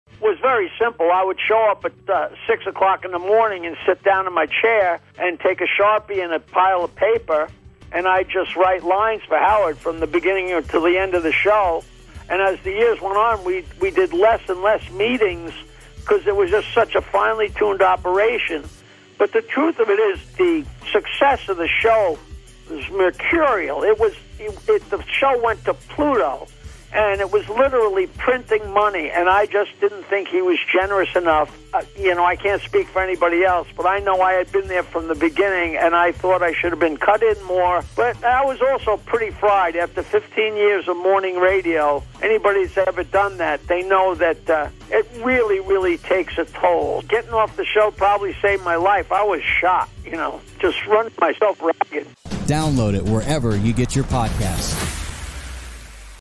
Jackie the Jokeman says Howard Stern wasn't generous enough and that is why he left the show
LISTEN TO THE FULL INTERVIEW AT https:/lnk.to/JokeMan

Jackie the Jokeman says Howard Stern wasn't generous enough and that is why he left the show when it was at its pinnacle of success